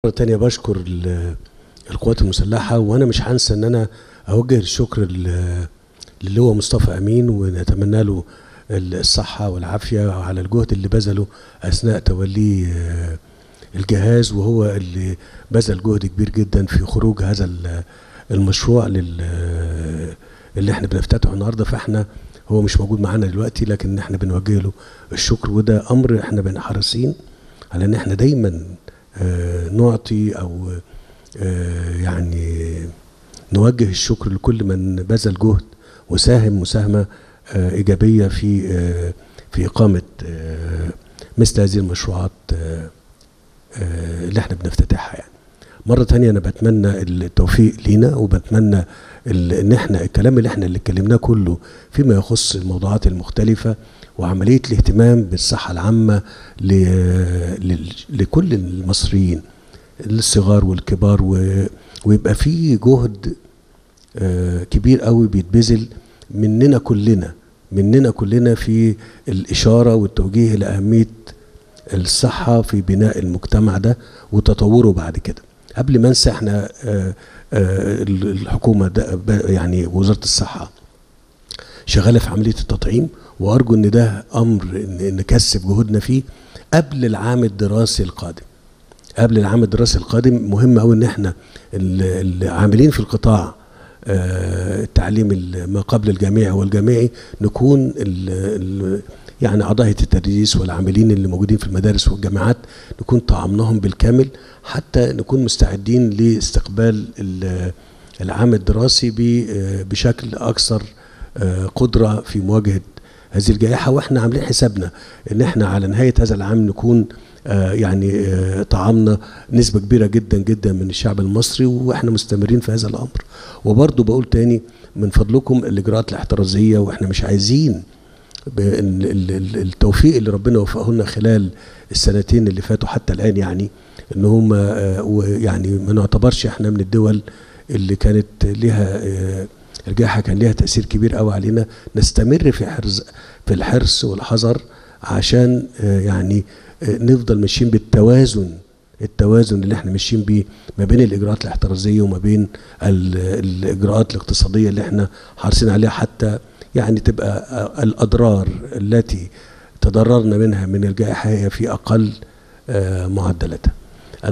الرئيس السيسي: بحلول نهاية هذا العام سيتم تطعيم الكثير من الشعب المصري
تغطية فعاليات اليوم 3 أغسطس 2021 للجولة التفقدية للرئيسالسيسى   في المدينة الصناعية الغذائية سايلو فودز.